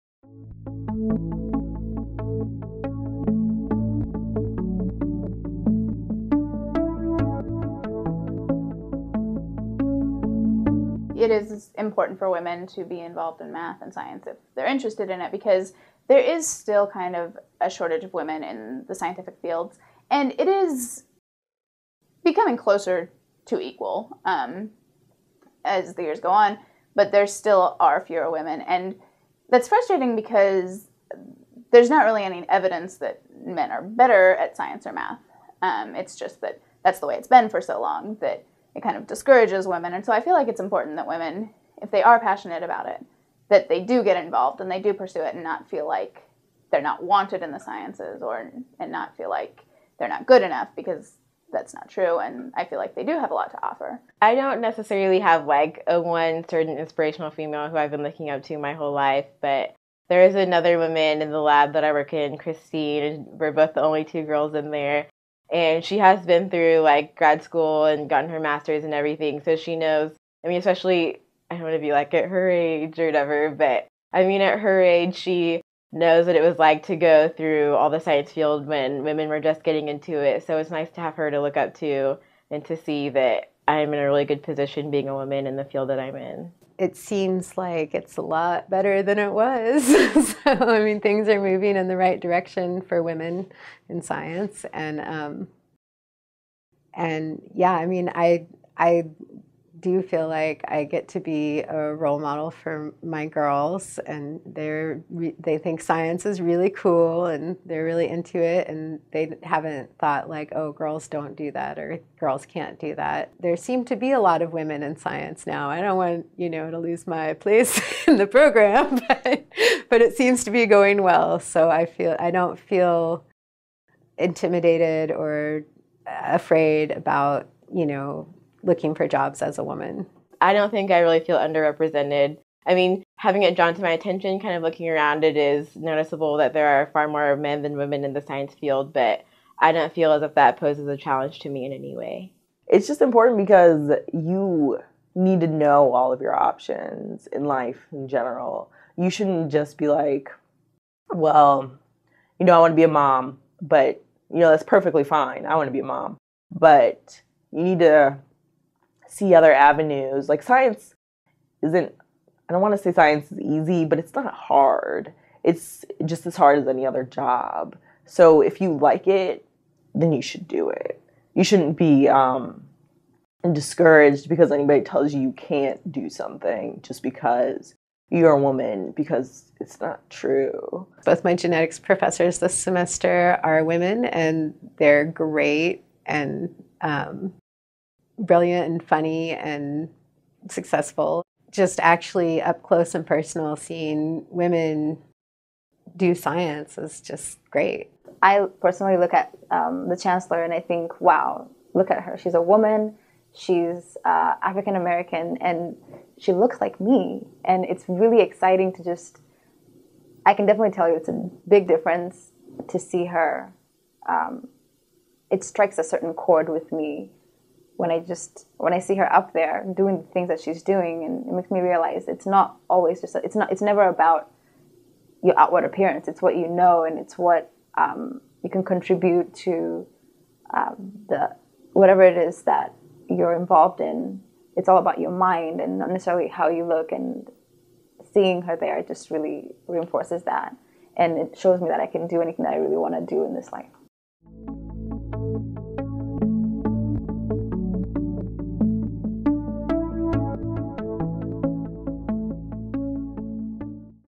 Women in Science and Math
Kelsy Kinderknecht, Kim Box, Terra Lubin, Akosua Kernizan, and Yvonne Kamau reflect on being a woman in science and math. They stress the importance of female role models who know how to balance school and family life.